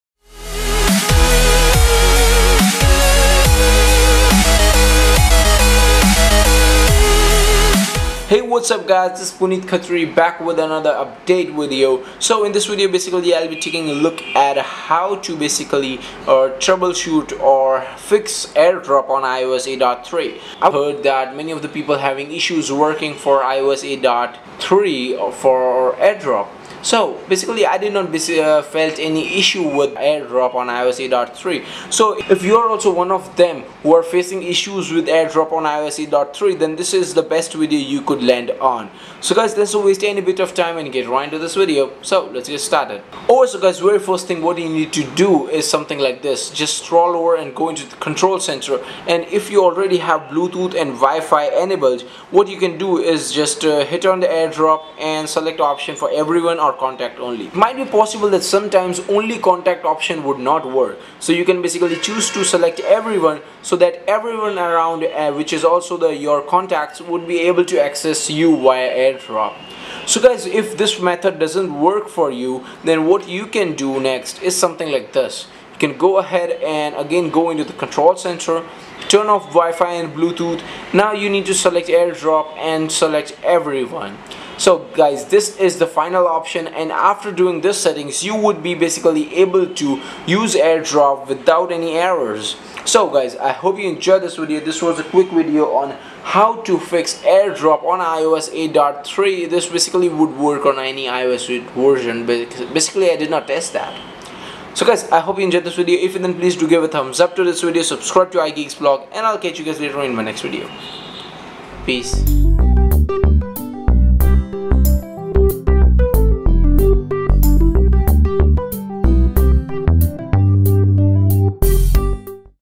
AirDrop Not Working in iOS 8 or Later on iPhone or iPad - Here is Possible Solution
Is Airdrop not working on iOS 8 or later. This video walks you through how to fix it.